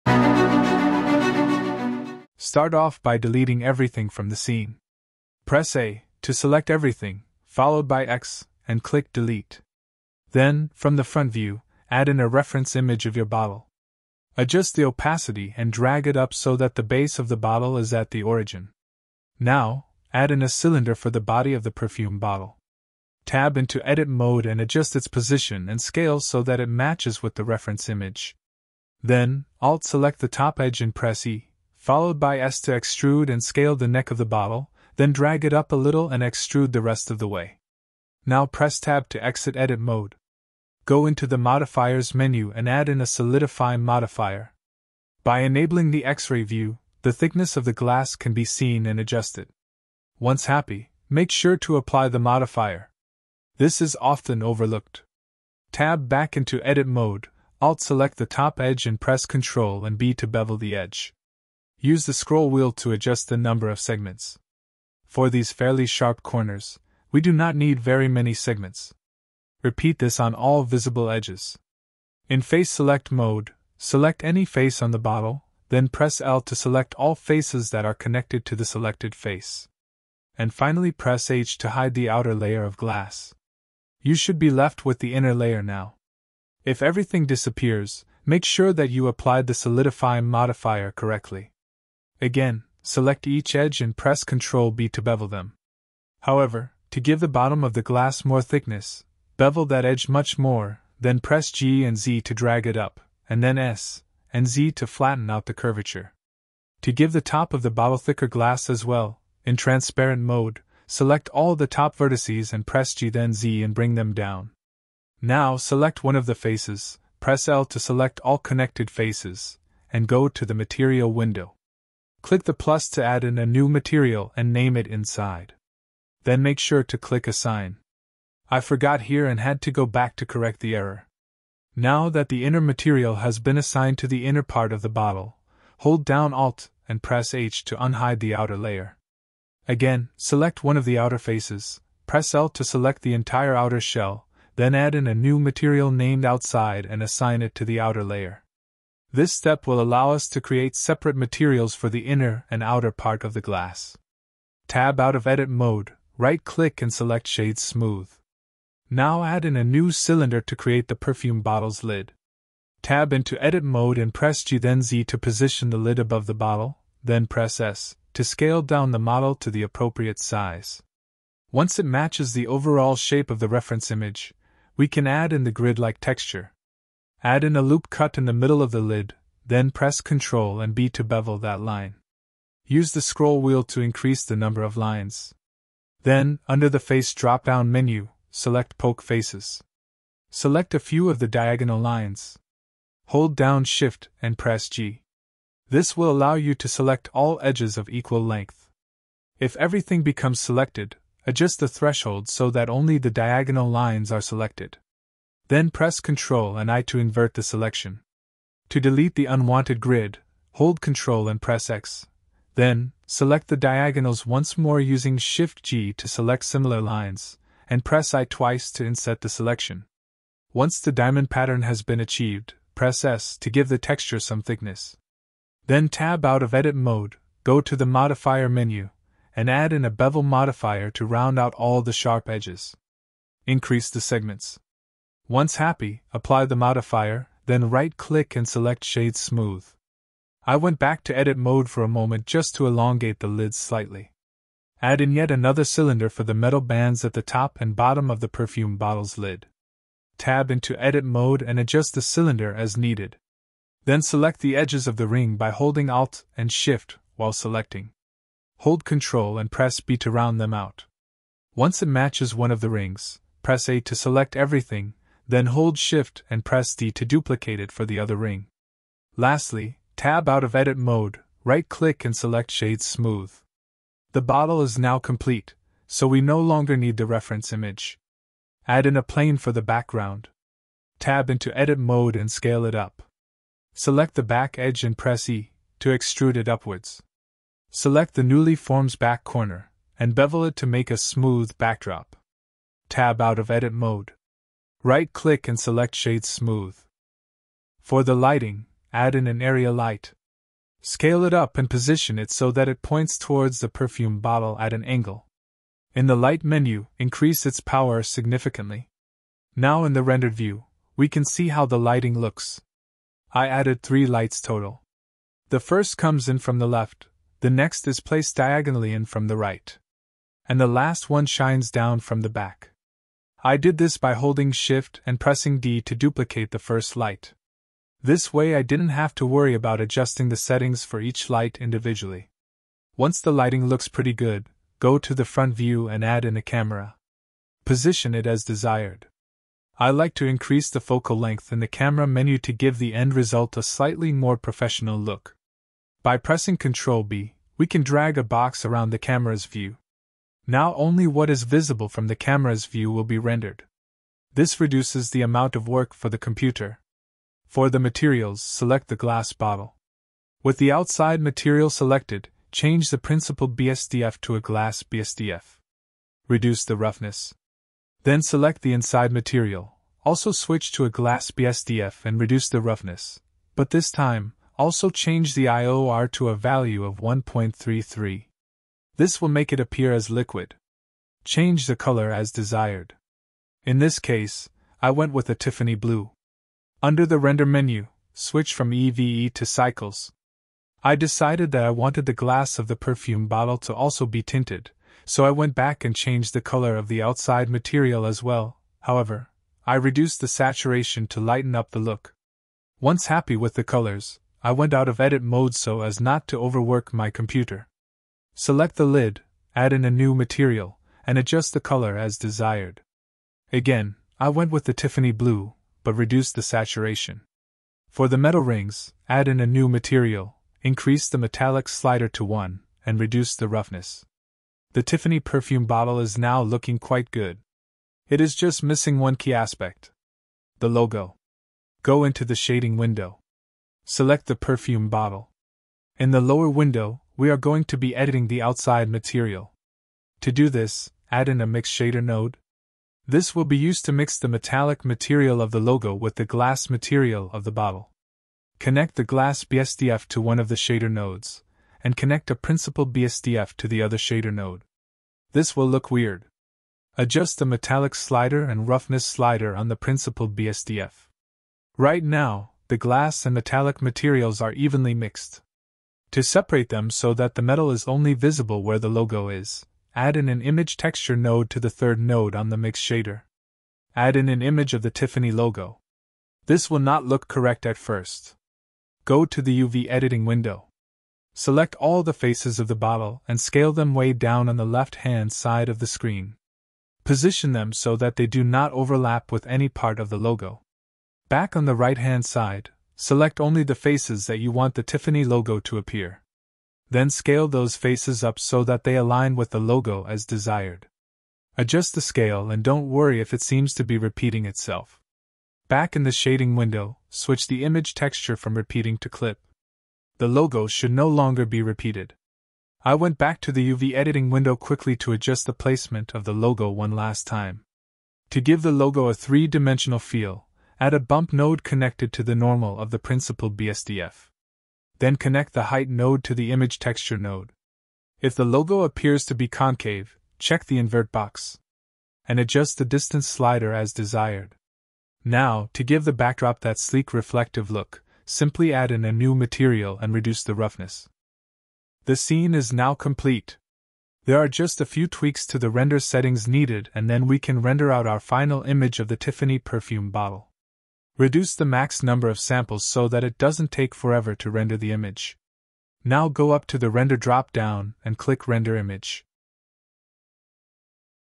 3D modeling in Blender: Tiffany & Co. Perfume bottle
This tutorial covers the start-to-finish process of creating a Tiffany and Co. perfume bottle in Blender.